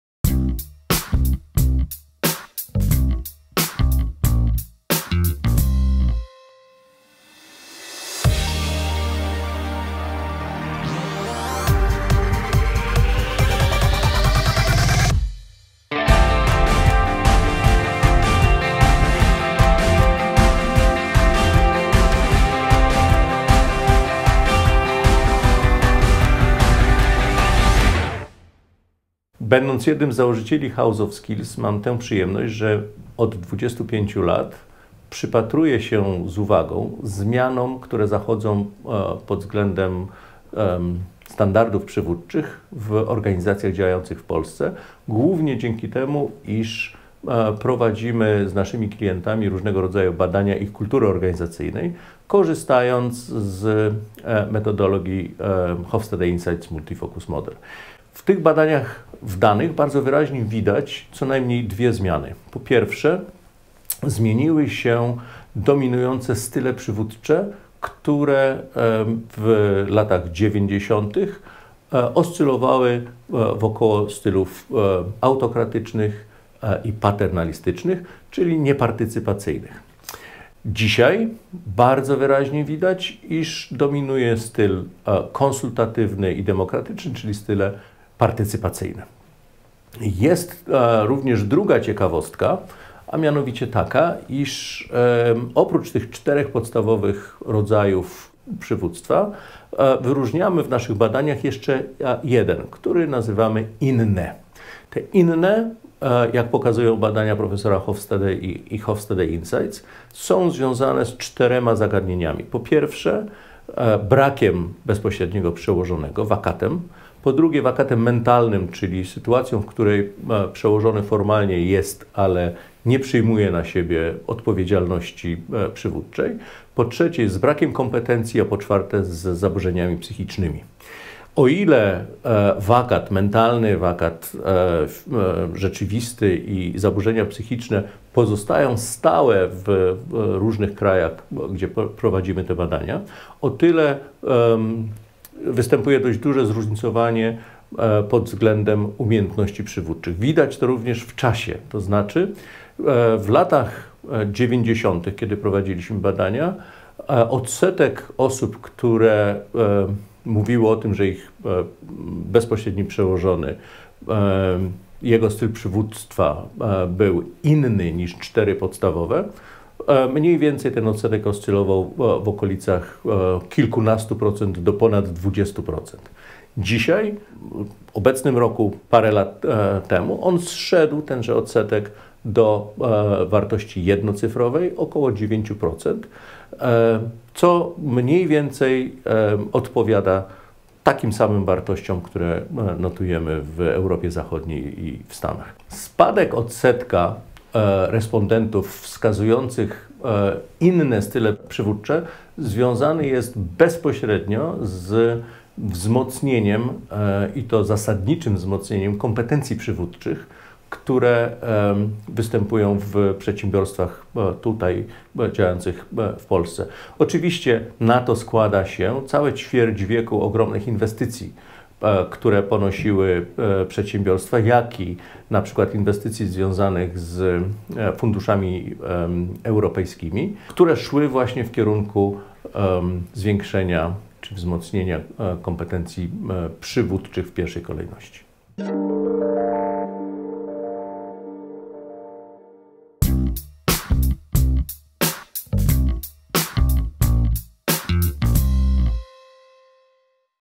Jak zmieniły się standardy przywództwa w ciągu ostatnich 25 lat?
Opinia eksperta House of Skills - Piotr Gryko